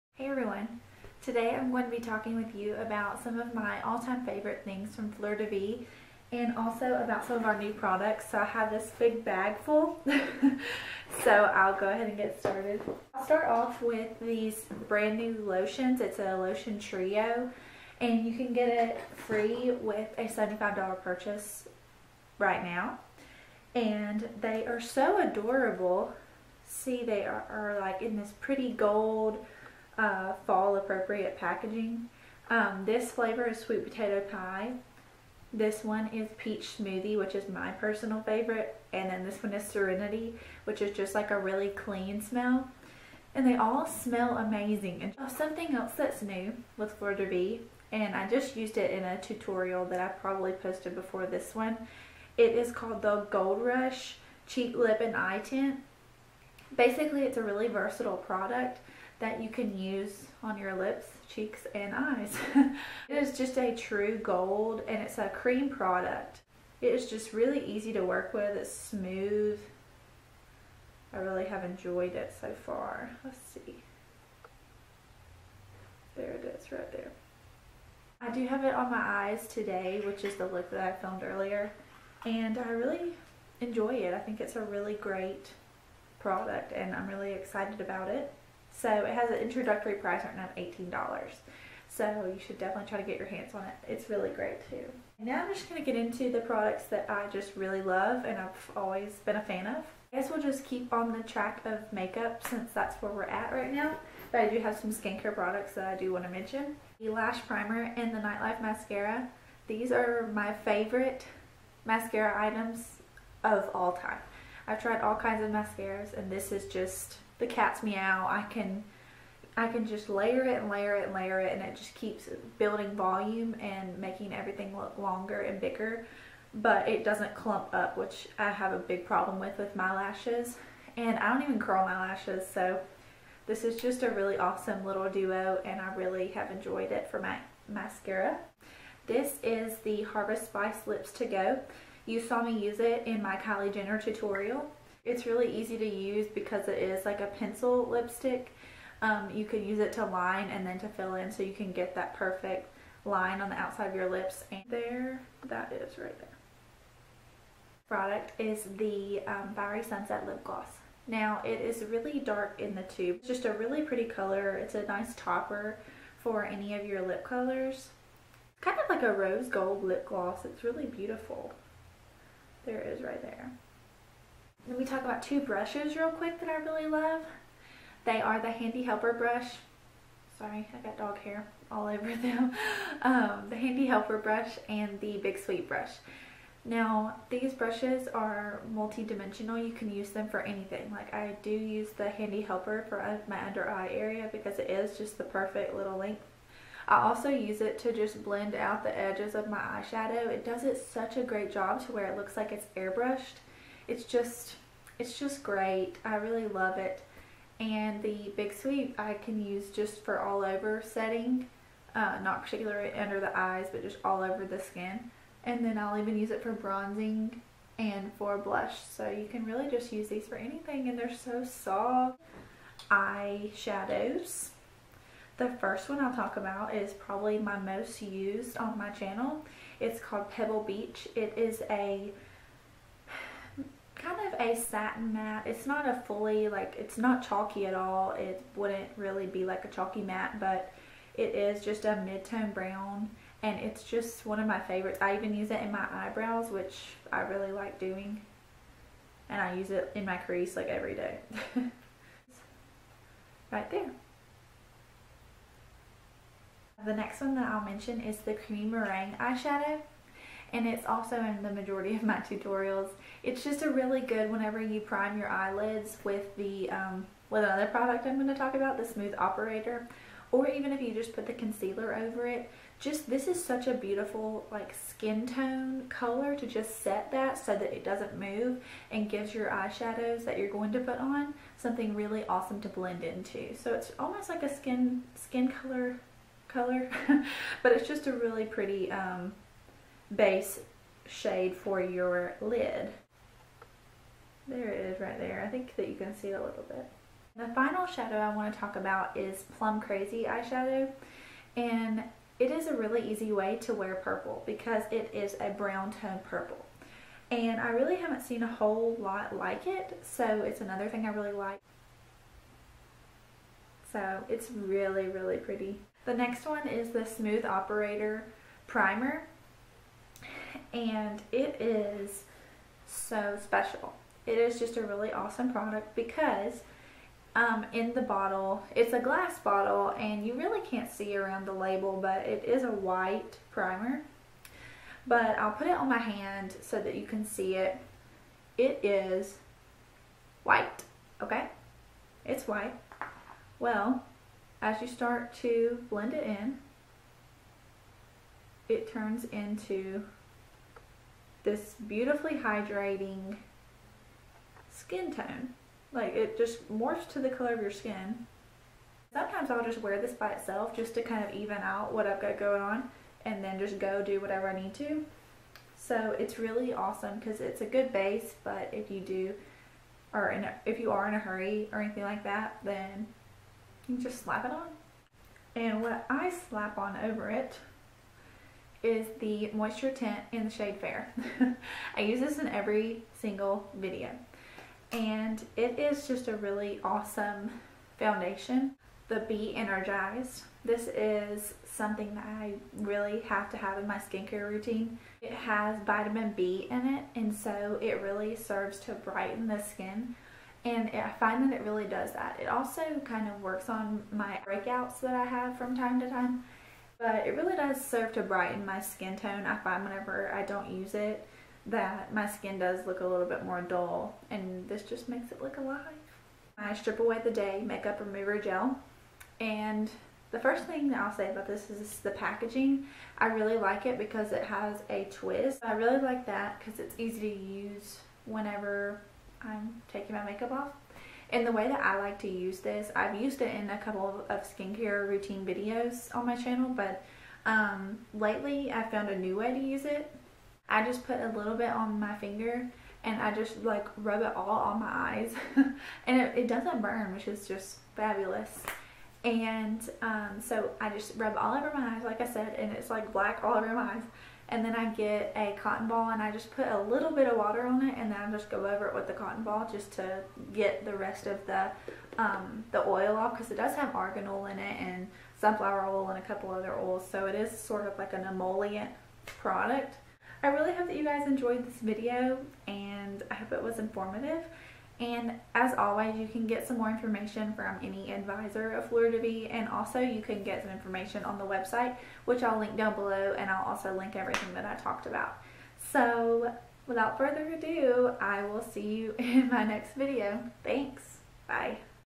All-Time Favorites and New Products | Fleur de Vie Beauty
These are my absolute favorite Fleur de Vie products.  What are your favorites?  Post them in the comments!

Products Mentioned:
Lotion Trio: Peach Smoothie, Serenity, and Sweet Potato Pie (FREE with a $75 purchase!)
Gold Rush Lip, Cheek, and Eye Tint

**I completely forgot to mention the new Sugar lipgloss!! I’ve been loving this natural gloss and you will too!  I will be posting a tutorial this weekend and I’ll use it!**